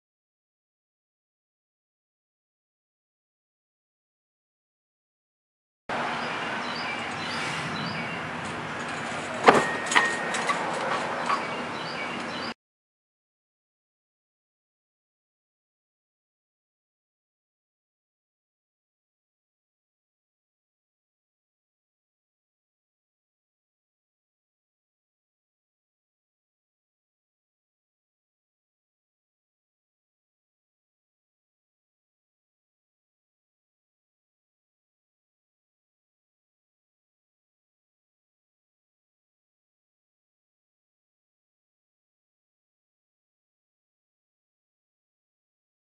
Inch DB Mini-tramp Training
Puling the 172. lb. Thomas Inch DB from the mini-tramp with no bouncing.
It would be interesting for me to see if anyone  who can fully DL the Inch could experiment with this and give me some feedback